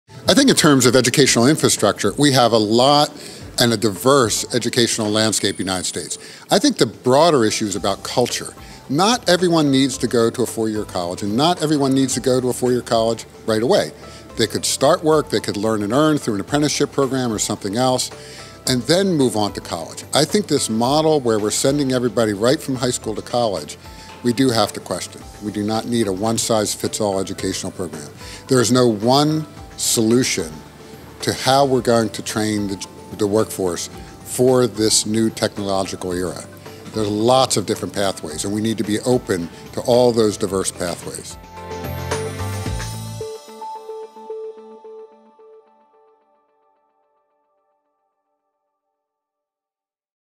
Patrick Harker Speaks at Tech Disruption Conference
“We have to stop viewing education as ‘one-size-fits-all,’” said Philadelphia Fed President Patrick Harker during a Technology-Enabled Disruption conference organized by the Dallas and Atlanta Feds May 24-25.